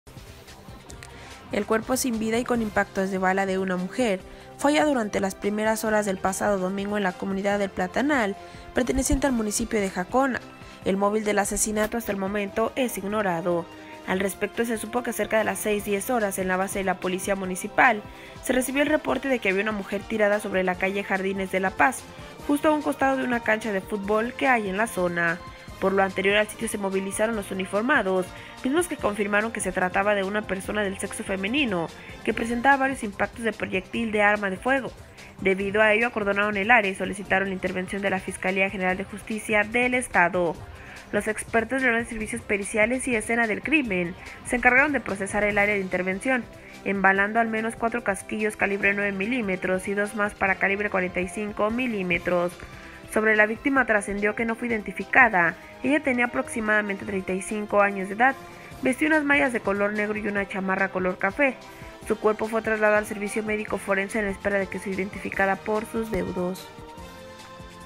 Encuentran a una mujer muerta a balazos en Jacona
+La víctima estaba tirada junto a una cancha de futbol en la comunidad de El Platanal

Jacona, Mich.- 06 de enero de 2020.-  El cuerpo sin vida y con impactos de bala de una mujer, fue hallado durante las primeras horas de este domingo en la comunidad de El Platanal, perteneciente al municipio de Jacona. El móvil del asesinato hasta el momento es ignorado.

AL respecto se supo que cerca de las 06:10 horas, en la base de la Policía Municipal se recibió el reporte de que había una mujer tirada sobre la calle Jardines de La Paz, justo a un costado de una cancha de fútbol que hay en la zona.

Por lo anterior al sitio se movilizaron los uniformados, mismos que confirmaron que se trataba de una persona del sexo femenino que presentaba varios impactos de proyectil de arma de fuego, debido a ello acordonaron el área y solicitaron la intervención de la Fiscalía General de Justicia del Estado.

Los expertos de la Unidad de Servicios Periciales y Escena del Crimen, se encargaron de procesar el área de intervención, embalando al menos cuatro casquillos calibre 9 milímetros y dos más para calibre 45 milímetros.

Sobre la victima trascendió que no fue identificada, ella tenia aproximadamente 35 años de edad, vestía unas mallas de color negro, y una chamarra color café, su cuerpo fue trasladado al Servicio Médico Forense en espera de que sea identificada por sus deudos.